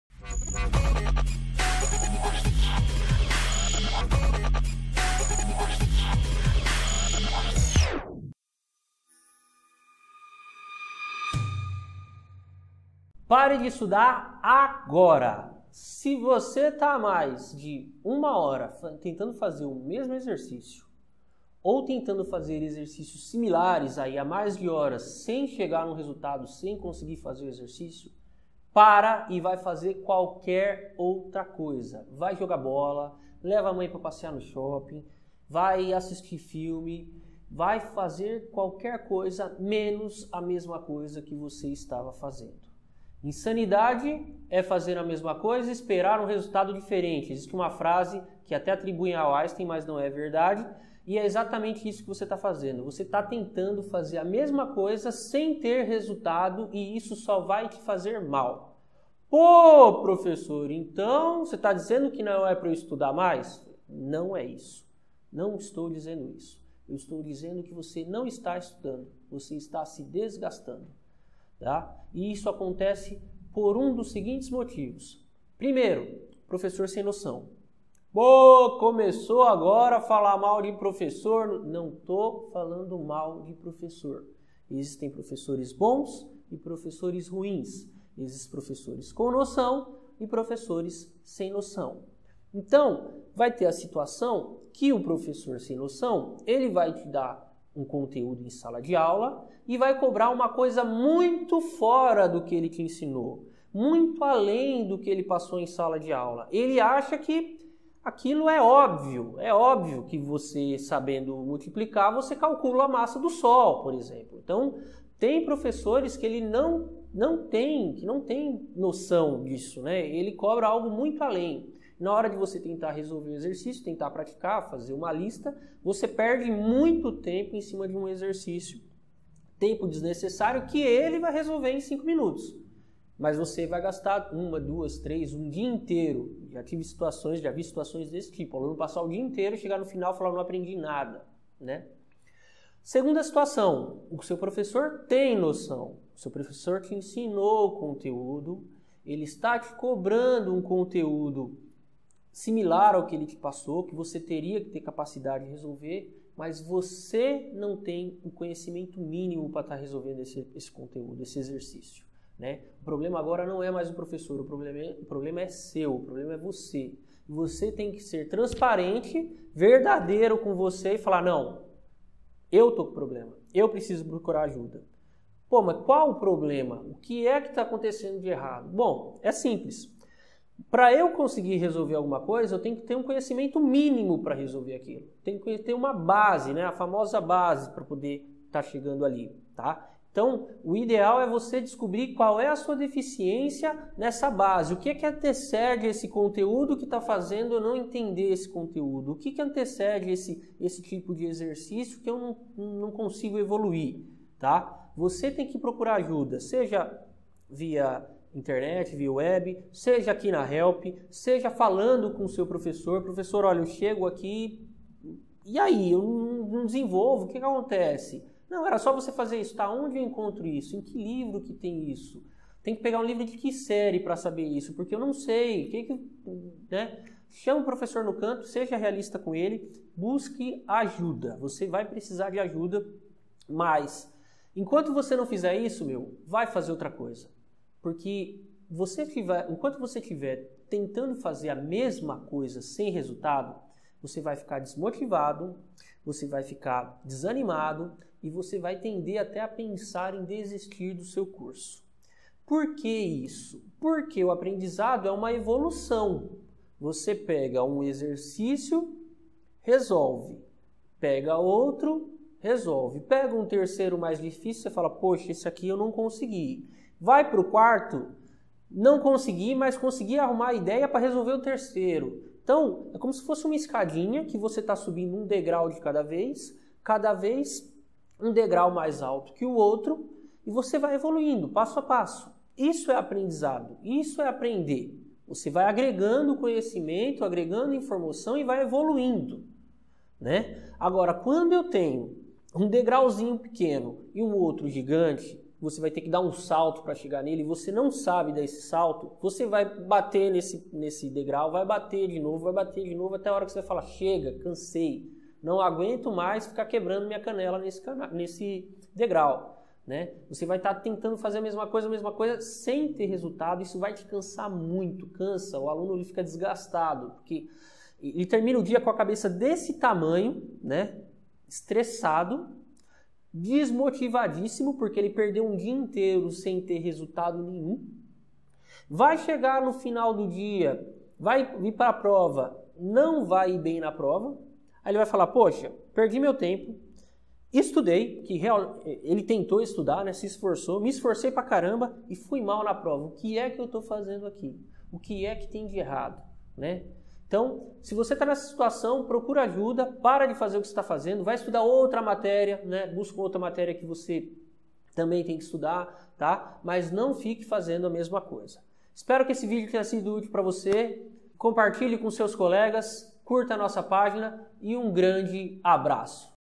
Pare de Estudar Agora!!
Olá pessoal, aqui é o Professor Dênis da Help Engenharia com mais uma superdica para vocês.Se você tem perdido muito tempo, estudando a mesma coisa e não aprende ou está tentando fazer um certo tipo de exercício e não consegue.
Segue essa dica do vídeo e deixe seu comentário aqui embaixo!
Espero que essa dica ajudem vocês, e em breve mais dicas, um grande abraço e bons estudos!